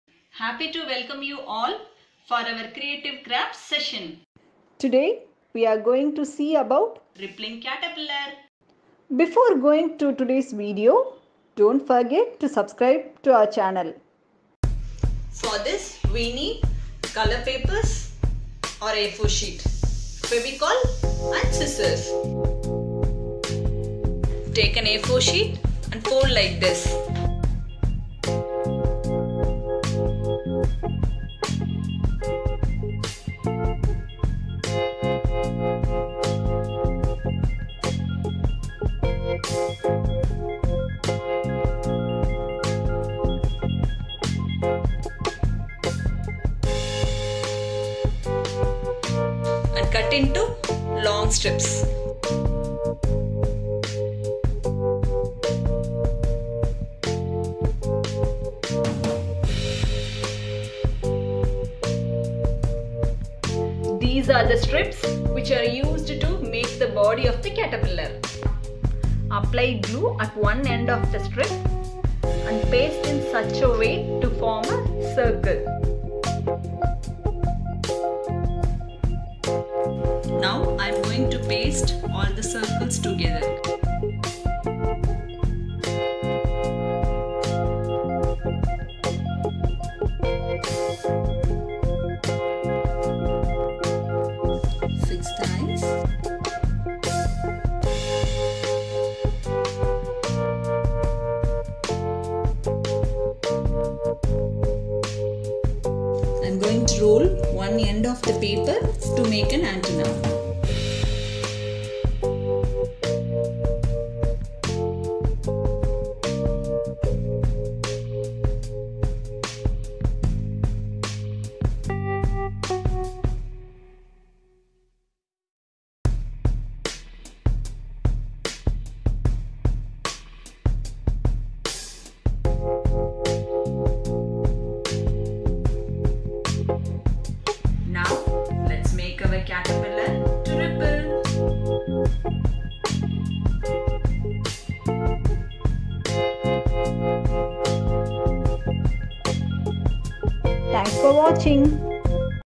Easy Paper Craft for kids| Rippling Caterpillar 🐛
Materials Used :-

* Colour Paper
* Scissors
* Glue
* Sketch

Our Similar Videos :-